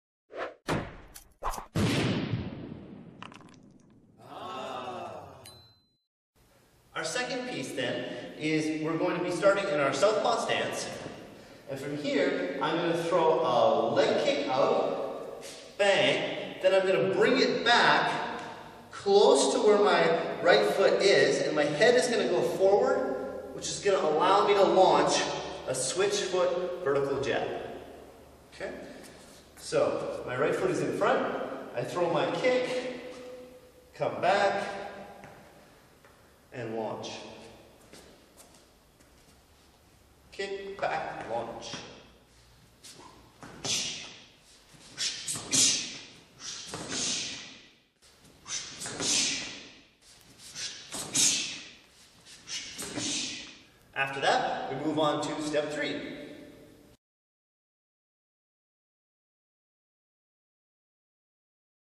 MMA made EASY: Dynamic Range 2 - Roundhouse to Switchfoot Vertical Jab
A great combo all on its own, this also fits nicely into the dynamic range combo